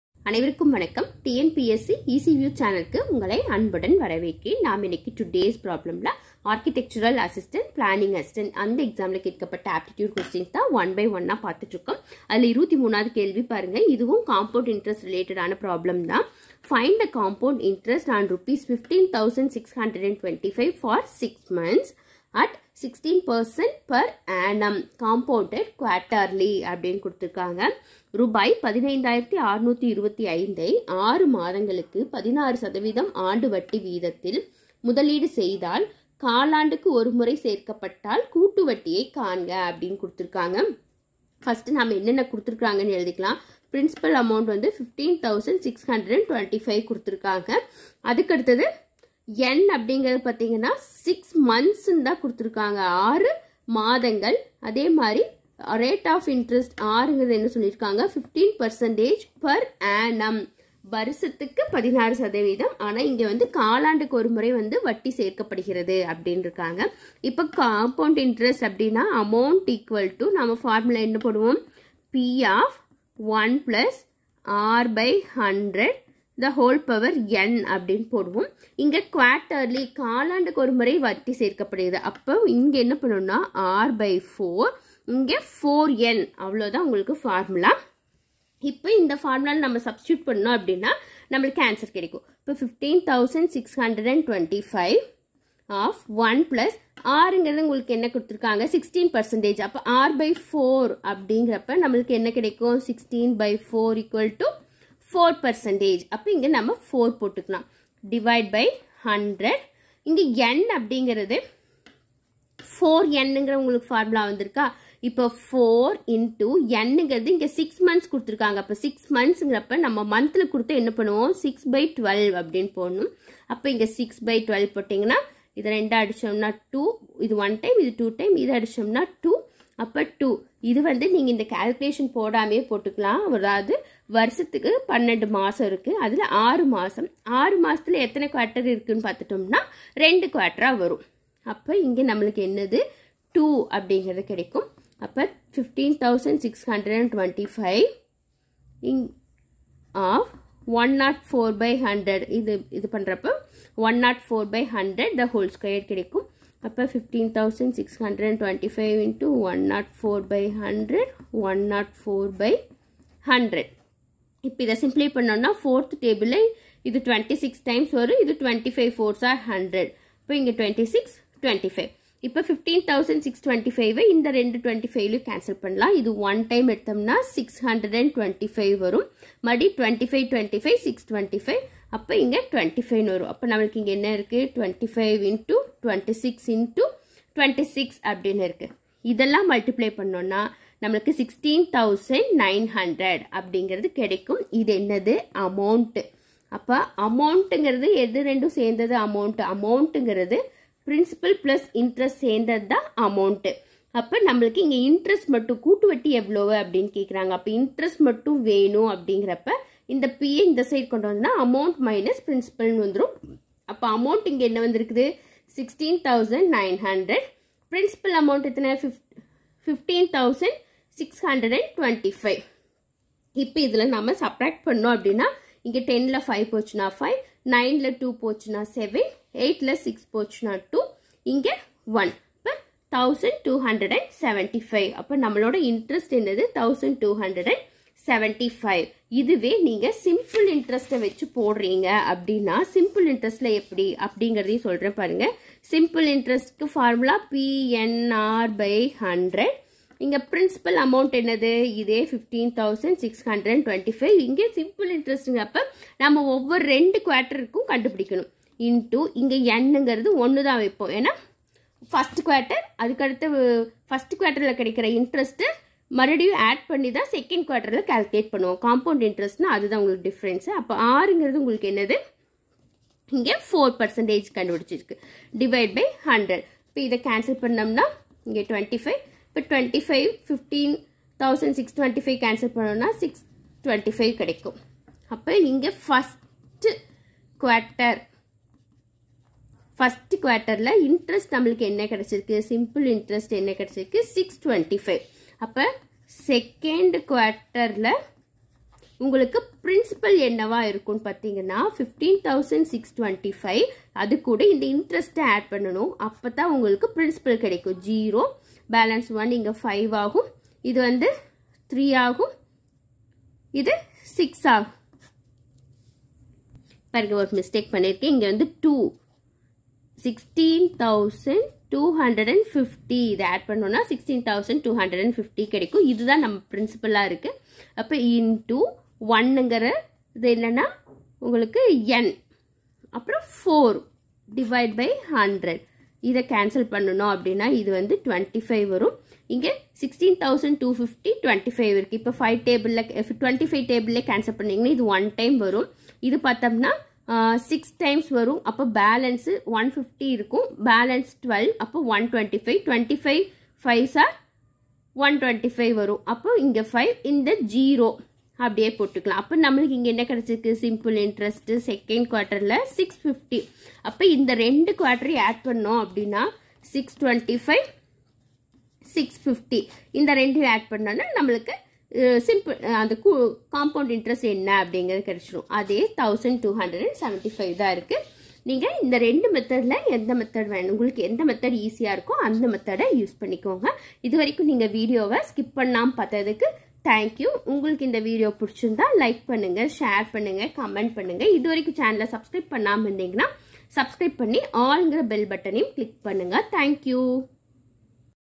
23)Compound Interest related problem|TNPSC easy view|Tangedco|TNUSRB|Tet|Group 2,2a,4|maths
In this video we see the following problem
23)Find the compound interest on ₹15625 for 6 months at 16% per annum compounded quarterly.

4) எதிர்ச்சொல்லை எடுத்தெழுதுதல்

5)பொருந்தாச் சொல்லைக் கண்டறிதல்

Official website for 1st to 12th Standard school books available

TNPSC easy view